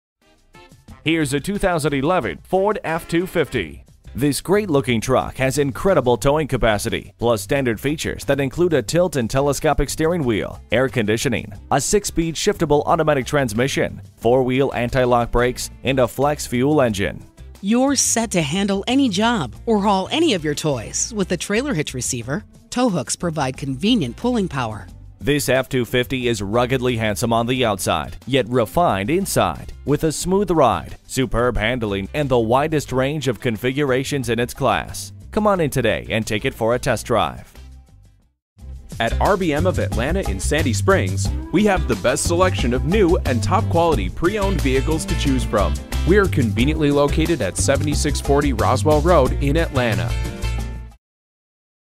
2011 Ford Super Duty F-250 SRW Atlanta GA Marietta, GA S1496B - SOLD
Year: 2011
Make: Ford
Model: Super Duty F-250 SRW
Trim: XL
Engine: 8 Cyl. 6.2L
Transmission: Automatic
Color: Oxford White
Interior: Steel
Mileage: 63291
Stock #: S1496B
VIN: 1FTBF2A60BEB21879
ONE OWNER, CLEAN CARFAX! AM/FM STEREO, 2 AUXILIARY POWER OUTLETS IN INSTRUMENT PANEL, 40/20/40 SPLIT BENCH FRONT SEAT, CENTER ARMREST, CUPHOLDER, STORAGE, STEERING WHEEL CONTROLS, TRAILER HITCH, TRUCK BED RACK, BLACK PAINTED STEEL FRONT BUMPER WITH GRAINED MOLDED-IN-COLOR TOP COVER AND BLACK LOWER VENT, MANUAL TELESCOPING TRAILER TOW MIRRORS WITH MANUAL GLASS, DUAL BEAM JEWEL-EFFECT HEADLAMPS, AND FRONT FENDER VENTS.

Address:
7640 Roswell Rd